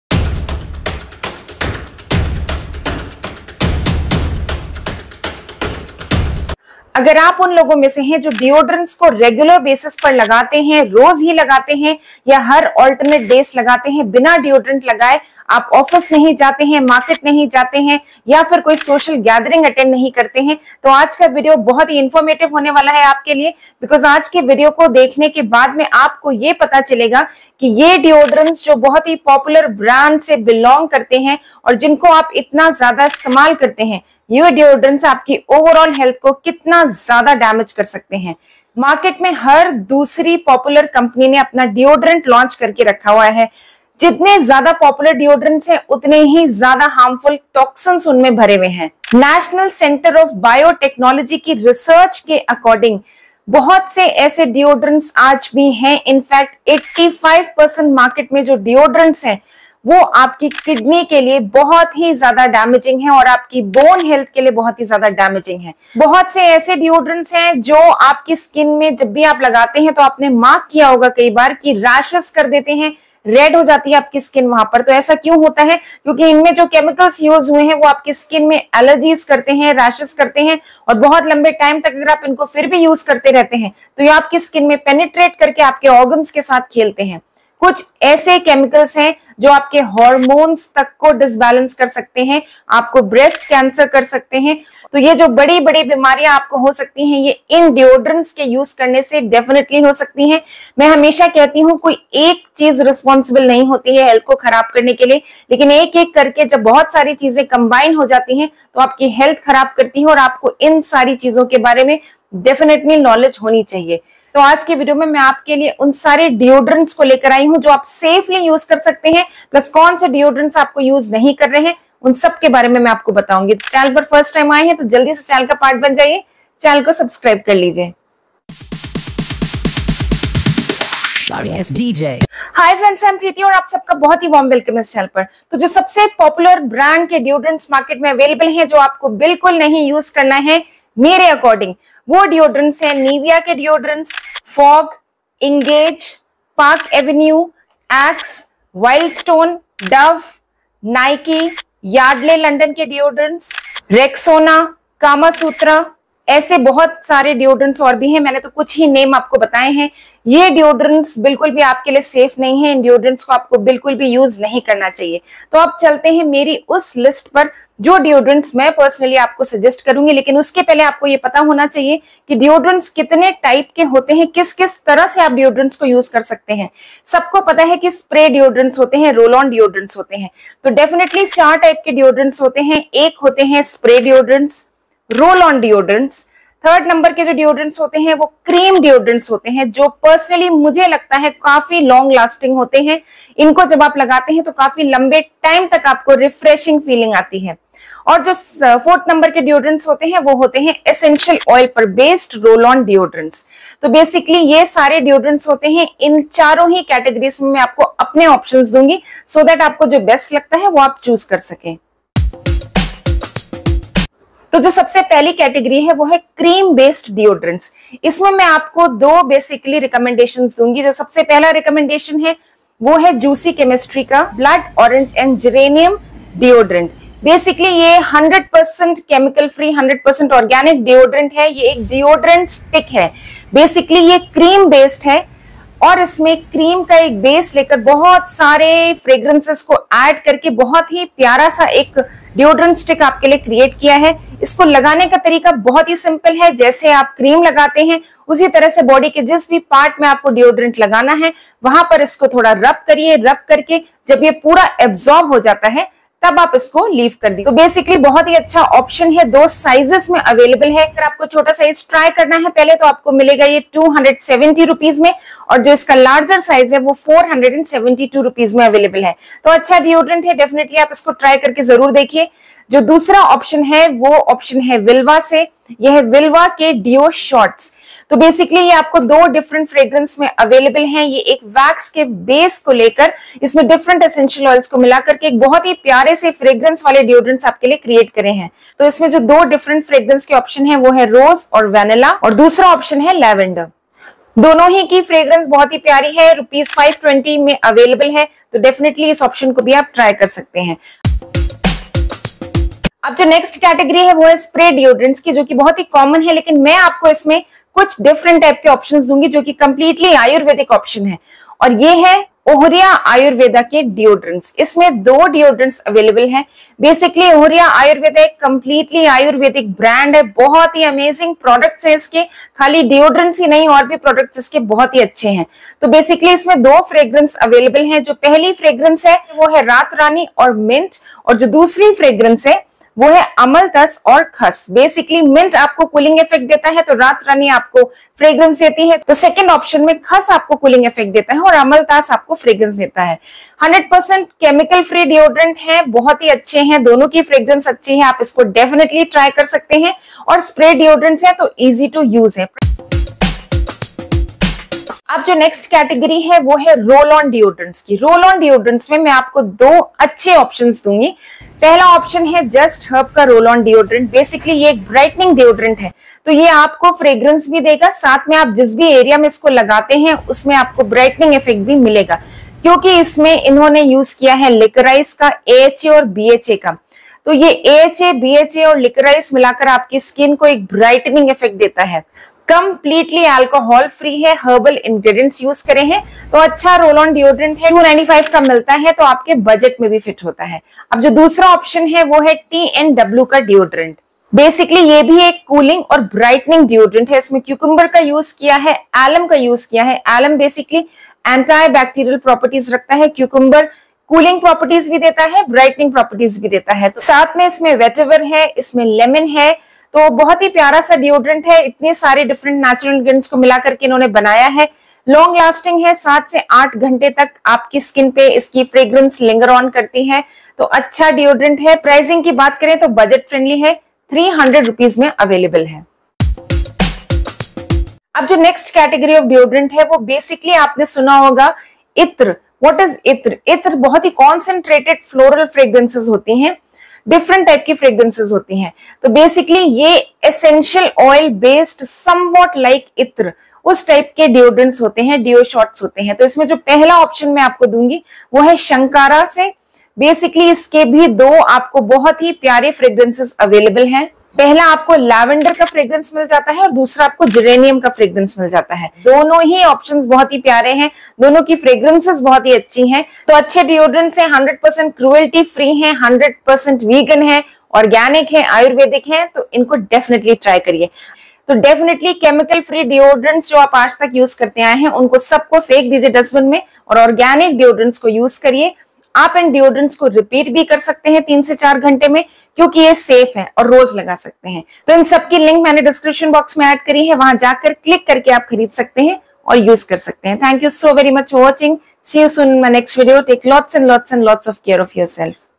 Top Organic Deodorants In India/Best Organic Deodorants/Toxin Free Safe Deodorants
Hi friends, in today's video I'm sharing  best organic Deodorants in India.These are Organically safe,chemical free Deodorants for body.These  can be used by men,women ,teenagers.

Links to buy
1
5
Just Herbs Under Arm Roll On for Women Floral Fiesta Long Lasting and Refreshing Women Deodorant - 50 ml
Best Deodorants in India
Safe Deodorants
Best organic Deodorants in India
Best Deodorants for men